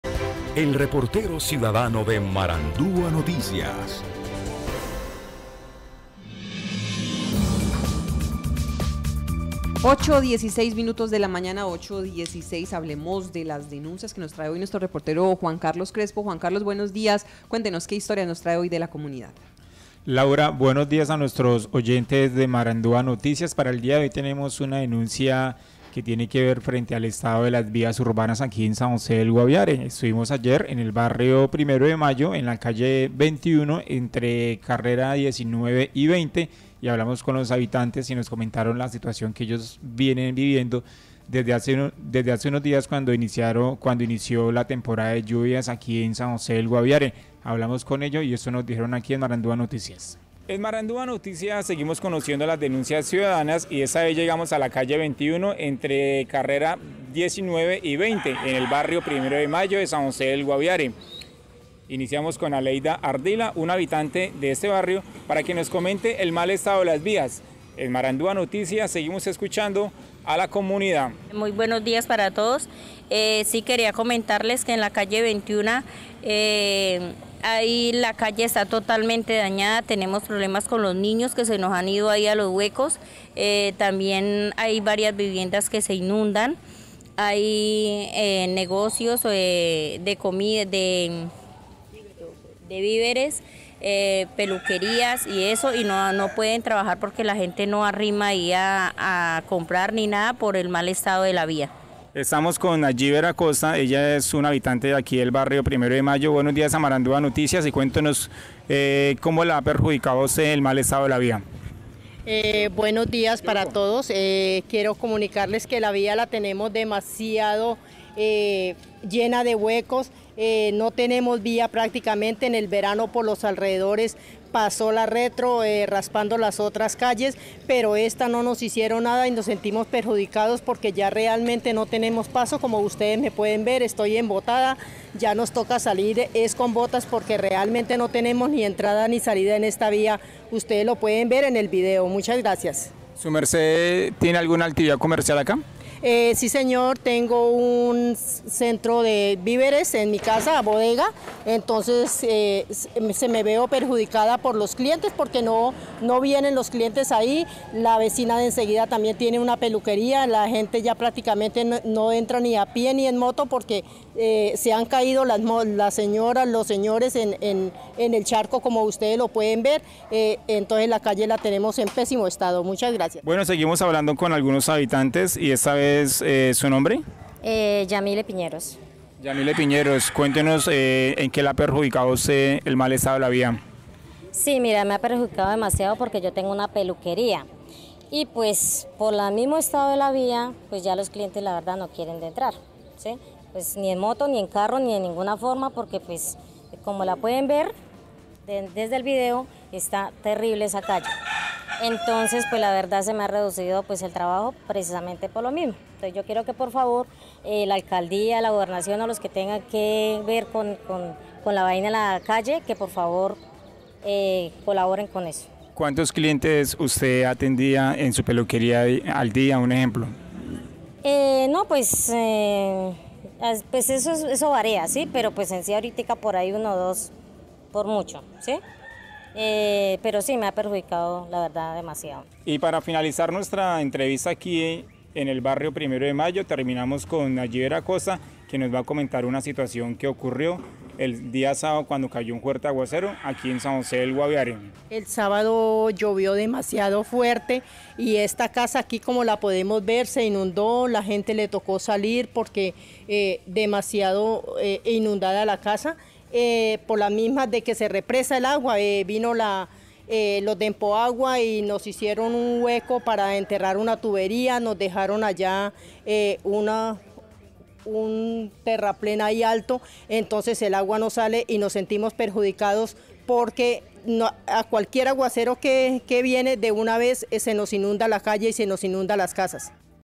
Denuncias ciudadanas en Marandua Noticias
MaranduaNoticias • El mal estado de la calle 21 entre carrera 19 y 20 en el barrio Primero de Mayo de San José del Guaviare tiene afectados a sus habitantes y pequeños comerciantes que dejaron de tener clientes por la falta de transitabilidad en la zona.